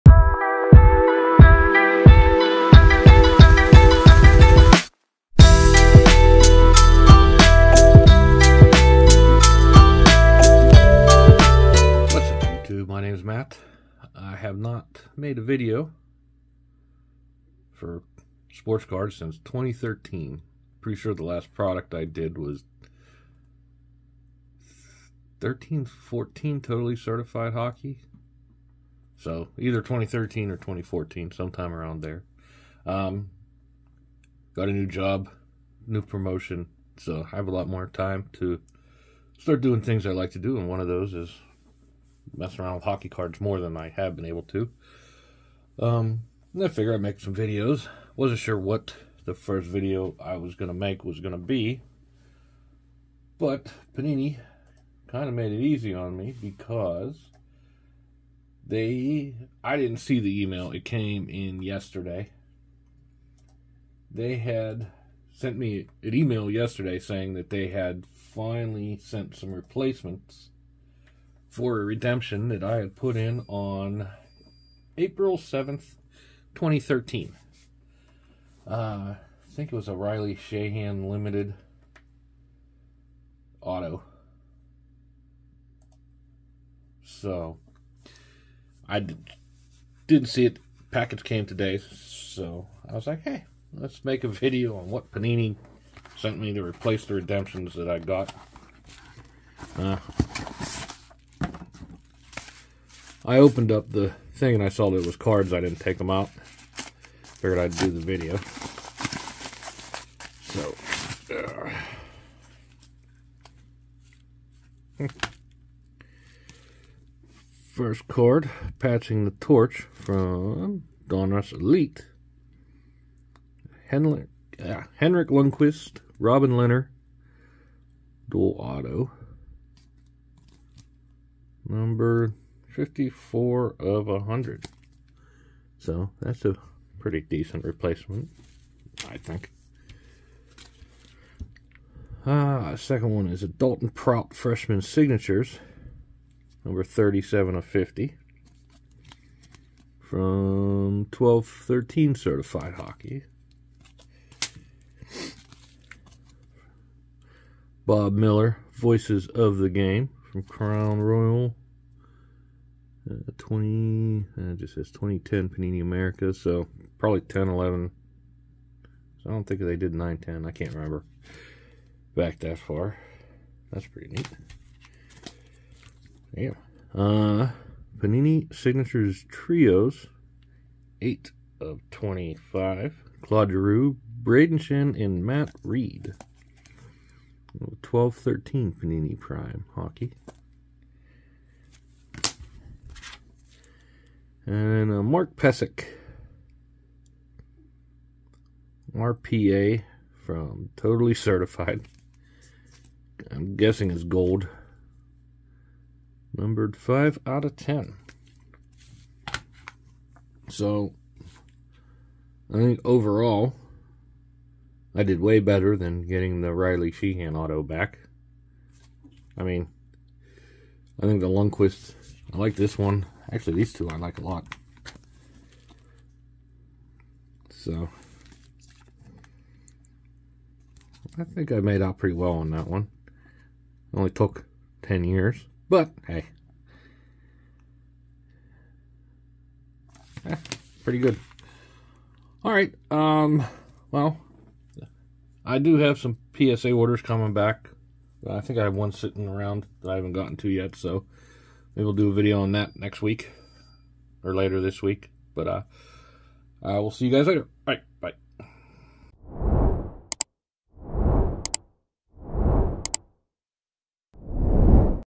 Panini Redemption 10 years in the making.
Put the redemption in on 04/07/2013.  They just sent the replacement redemptions back yesterday.